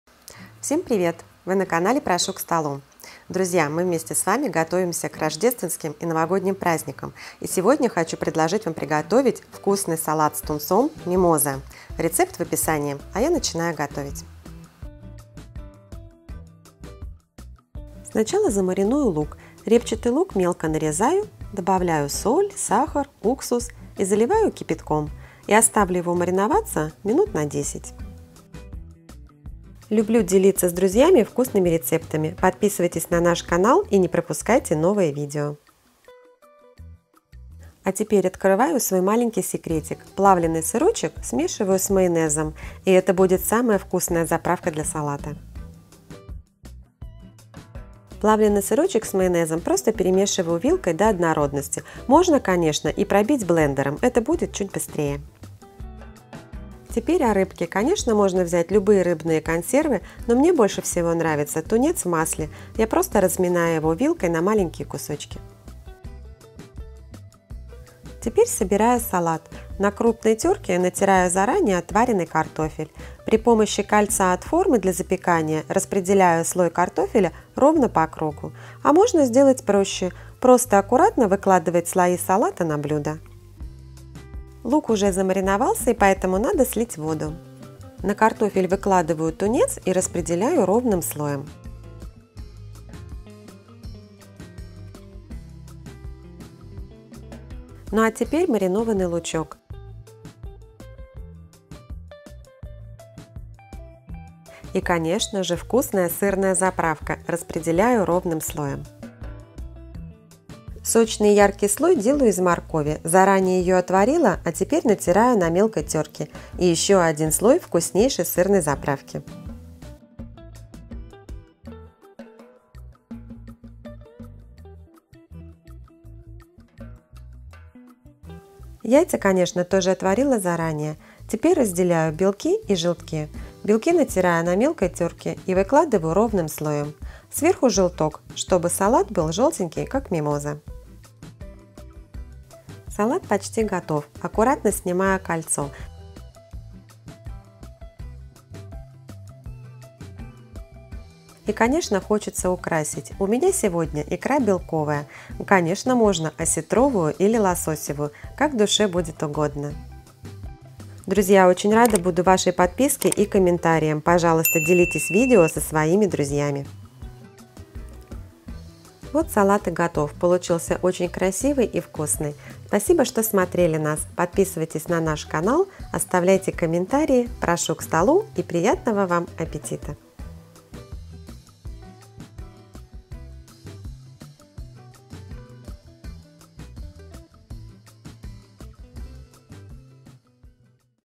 Салат "МИМОЗА" Классический Рецепт с Секретиком! Салат на Праздничный стол 2021.
Салат "МИМОЗА" Классический Рецепт с Секретиком! Этот салат украсит ваш праздничный стол и готовится очень легко и просто.

Рецепт:
1 баночка тунец в масле
3-4шт.  картофеля
2 моркови
4 яйца
50 гр плавленый сырочек
4-5 ст. л. майонеза
1 шт. лук репчатый (маринад-1ч.л сахара, ½ ч.л соли, 2ч.л уксус 5-9%)

Приготовление:

Картофель, морковь, яйца отворить до готовности и остудить.
Лук порезать мелким кубиком, добавить соль, сахар, уксус и залить кипятком. Оставить мариноваться на 10 мин.
Заправка: плавленый сырочек смешать с майонезом.
Собираем салат слоями: натертый на терке картофель, тунец, маринованный лук, заправка, морковь натертая на терке, заправка, белки, желтки.